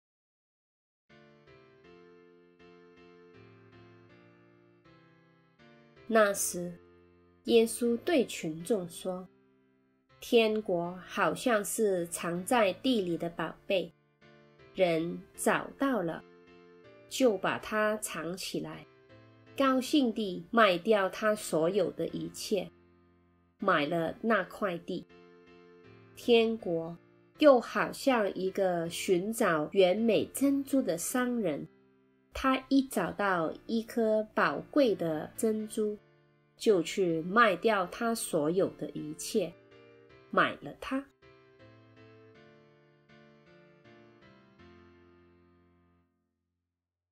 常年期第十七週 星期三🍀普通話〖瑪竇福音 13:44-46〗
瑪竇福音 13:44-46（寶貝和珍珠的比喻）

那時，耶穌對群眾說：「天國好像是藏在地裏的寶貝；人找到了，就把它藏起來；高興地賣掉他所有的一切，買了那塊地。天國又好像一個尋找完美珍珠的商人；他一找到一顆寶貴的珍珠，就去賣掉他所有的一切，買了它。」